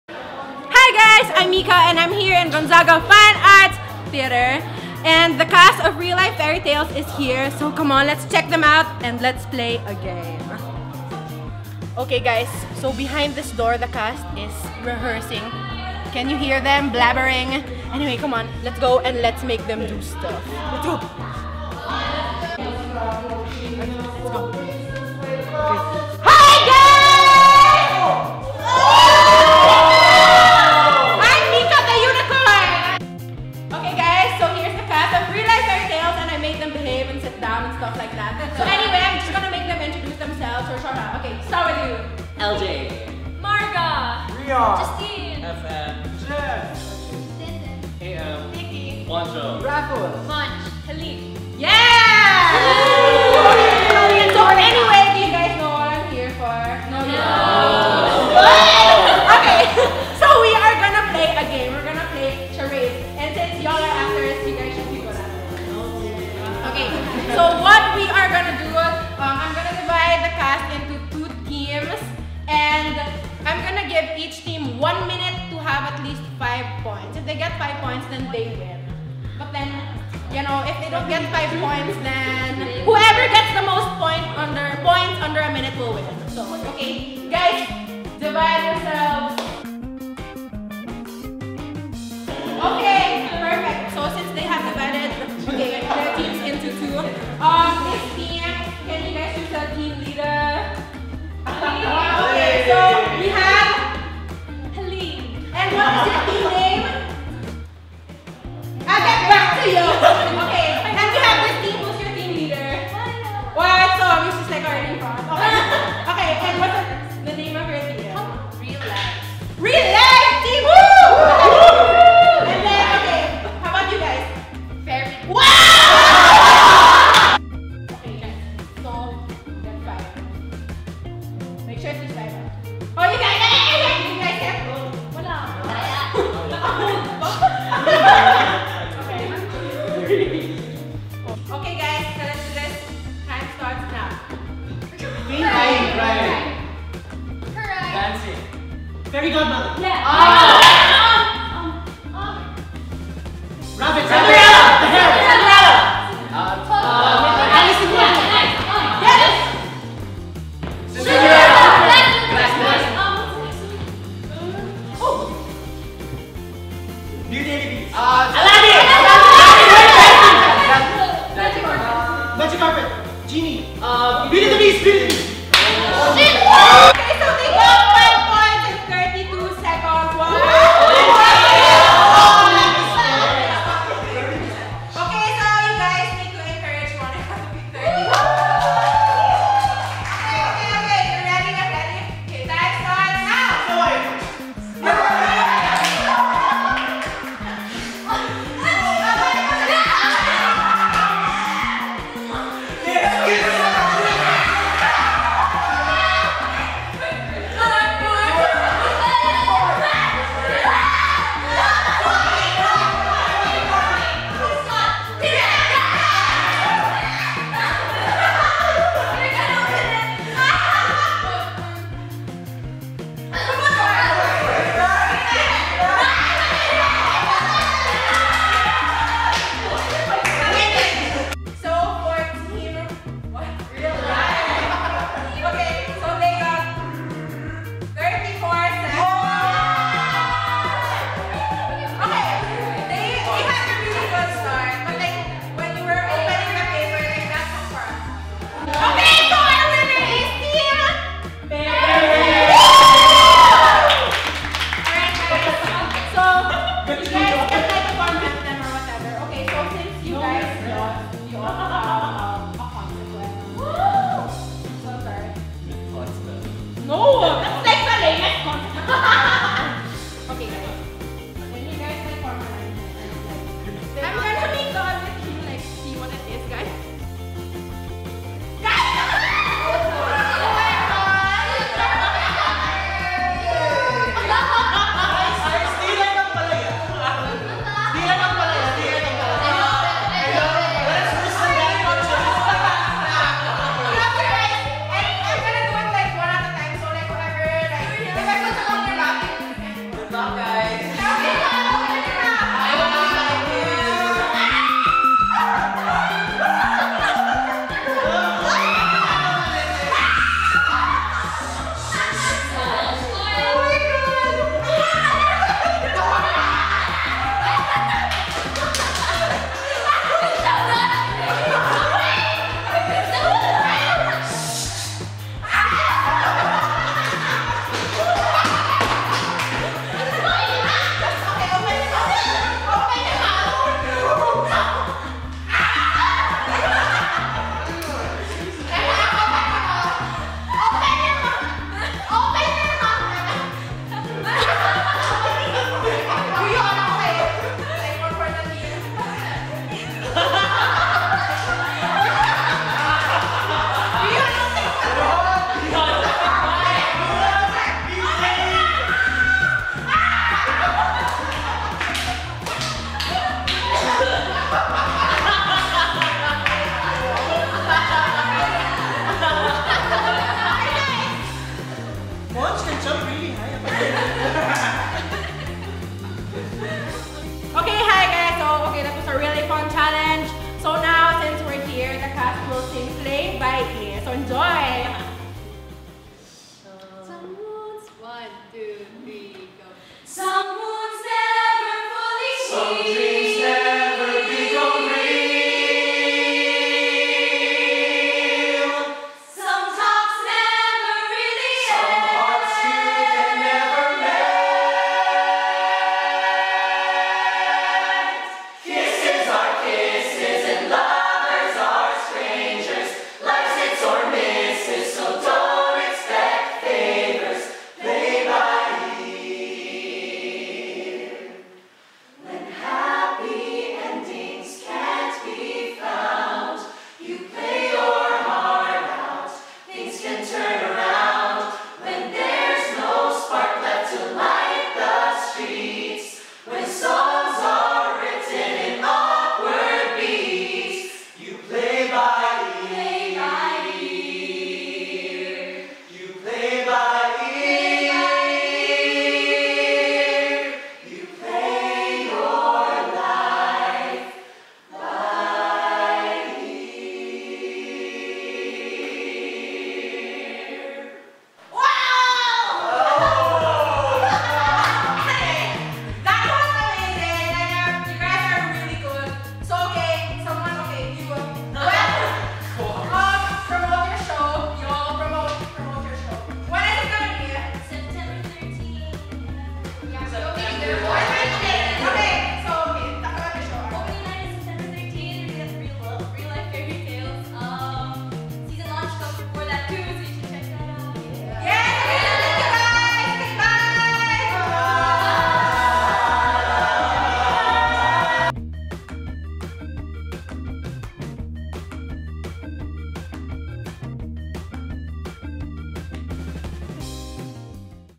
CastChallenge: blueREP's Real Life Fairytales
What happens when a unicorn visits the land of Real-Life Fairytales? CHARades, of course!

See the cast of blueREP's Real-Life Fairytales take on a challenge even the production can't compare to and enjoy a sneak peek of "Play by Ear"!

Videos and Editing by Twinkle Liboon
Special thanks to Luigi Soriano, Niña Bermio, and Steph Escuadro
Spiel by Bea Racoma